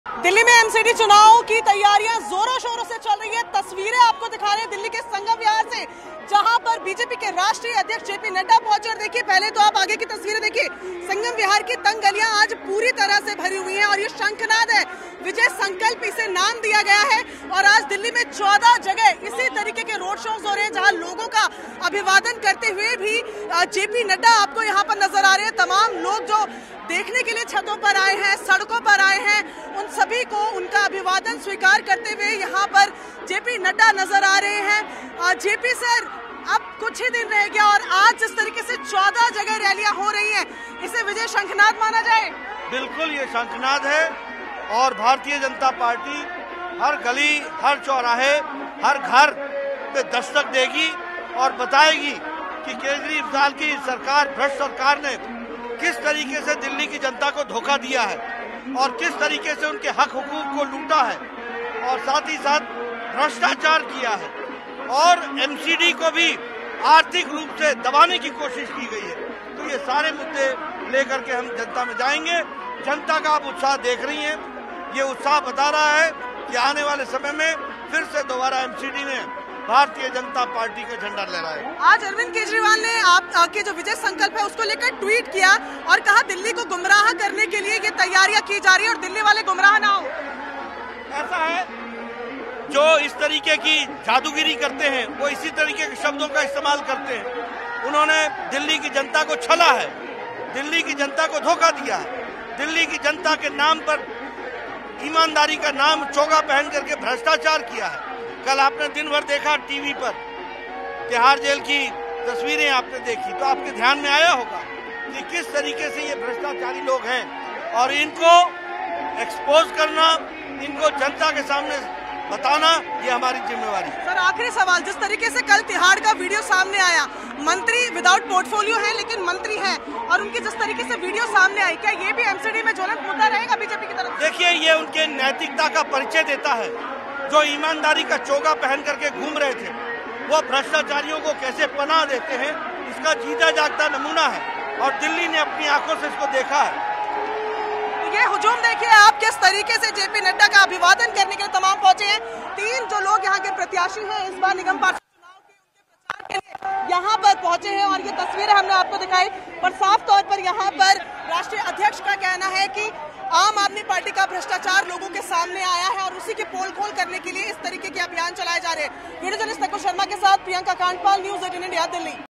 Delhi MCD Elections: दिल्ली में नगर निगम चुनाव के प्रचार पर मैदान में उतरे जेपी नड्डा ने किया रोड शो
भारतीय जनता पार्टी के राष्ट्रीय अध्यक्ष जे. पी. नड्डा ने संगम विहार से रोड शो किया।